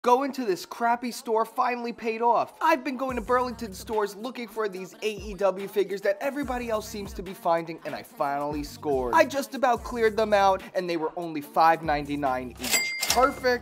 $5 Aew action figures at Burlington! Wow!
I’ve been in more Burlington stores that any person should ever go to 🤮🤣🥲 but it finally paid off! Found a fresh stock of Aew action figures from @jazwares and found most of the ones I was looking for. Full toy hunt will be on my YT soon!